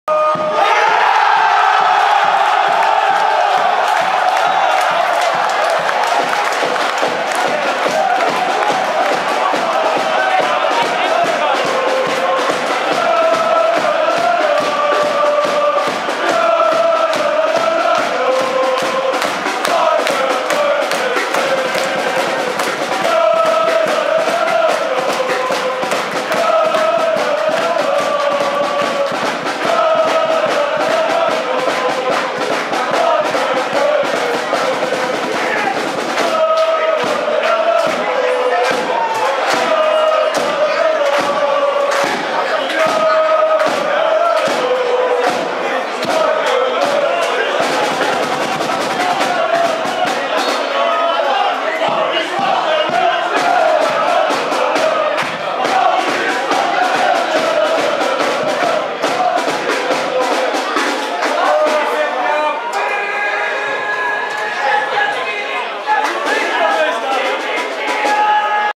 Spakenburg - Kozakken Boys ( 2-0 ) 8-3-2014 | 2-0 Van der Wilt
De wedstrijd tussen SV Spakenburg en Kozakken Boys in de Topklasse zaterdag